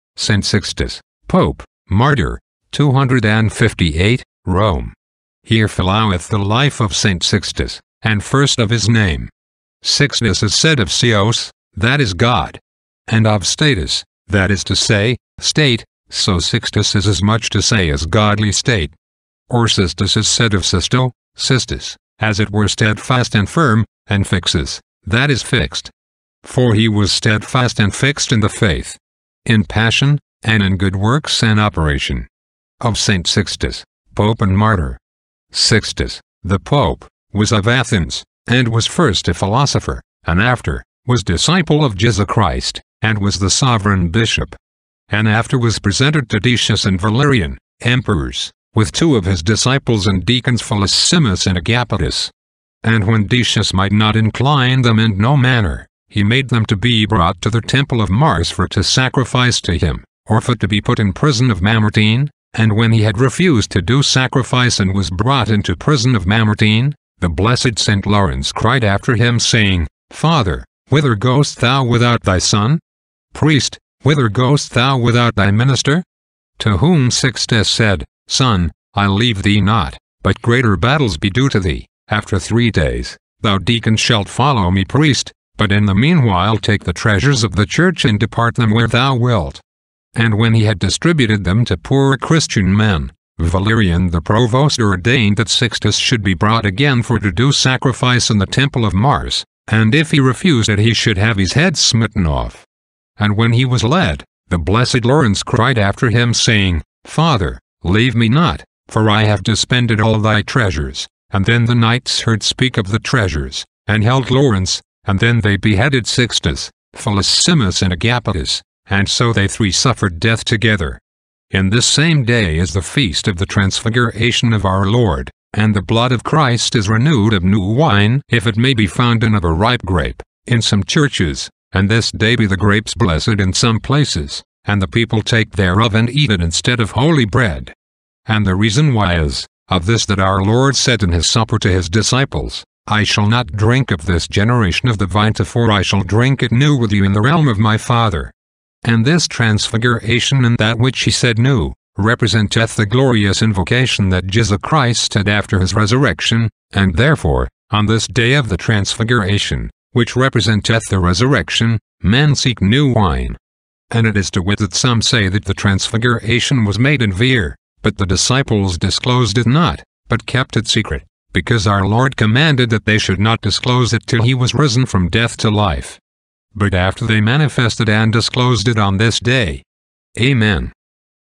Aug 06 - Saint Sixtus II - Pope - Martyr - 0258 - Rome
From the Legenda Aurea Sanctorum.